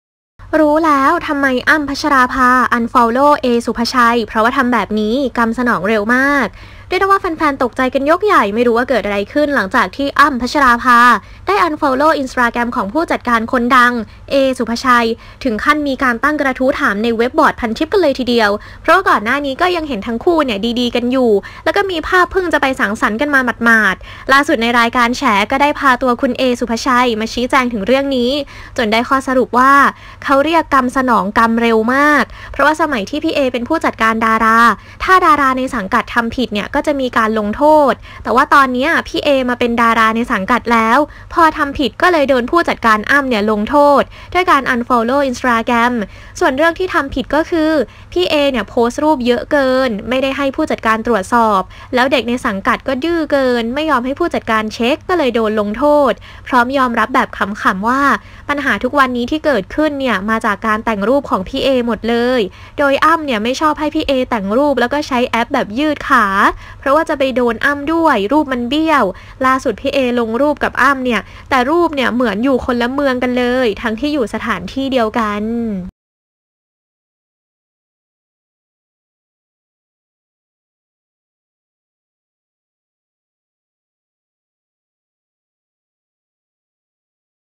รู้แล้ว ทำไม ‘อั้ม พัชราภา’ อันฟอลโล ‘เอ ศุภชัย’ เพราะทำแบบนี้ กรรมสนองเร็วมาก
รับชมทั้งเพลย์ลิสต์